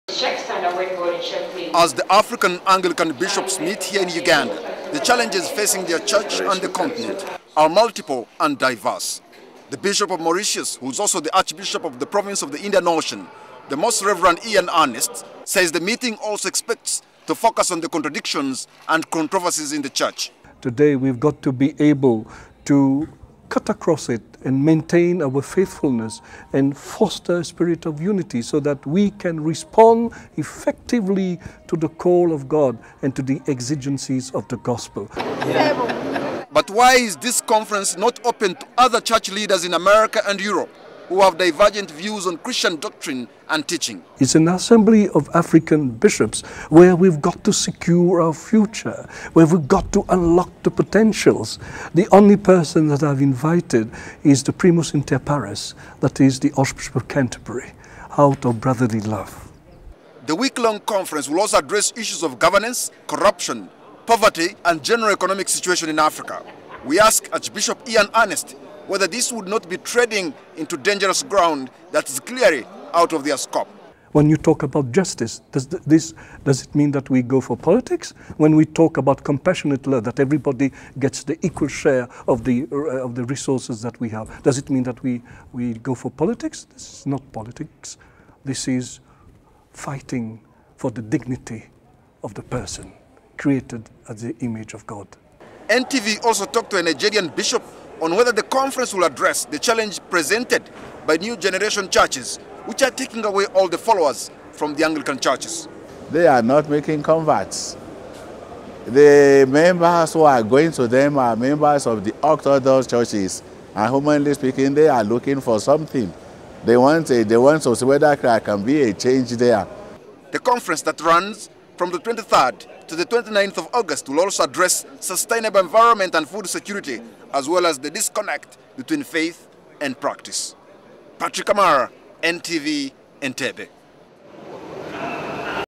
African Anglican Bishops meet starts
Bishops of the Anglican Church in Africa have gathered here in Uganda to begin a week long Conference on the Challenges facing the leadership of the Church on the Continent.